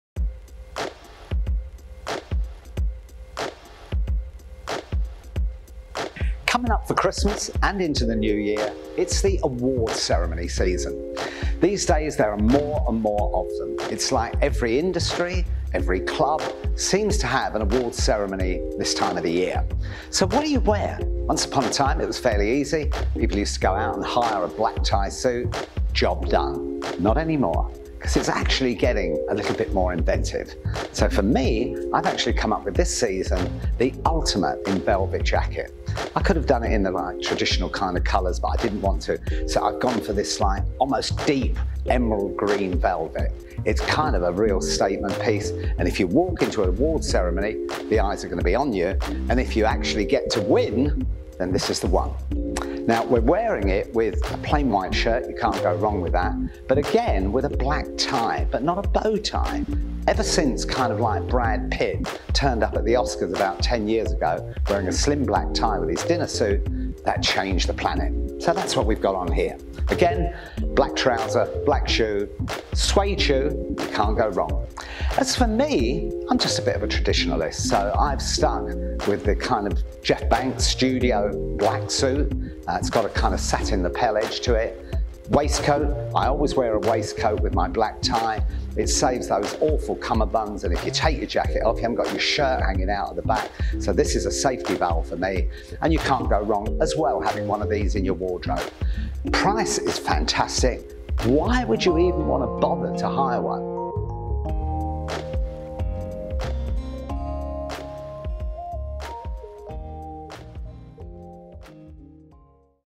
What to Wear to an Award Ceremony | Jeff Banks Lookbook
Jeff Banks and Hilton line up the perfect award ceremony wardrobes, with a range of black tie looks and formal wardrobes with extra designer flair. Only from the Jeff Banks look book.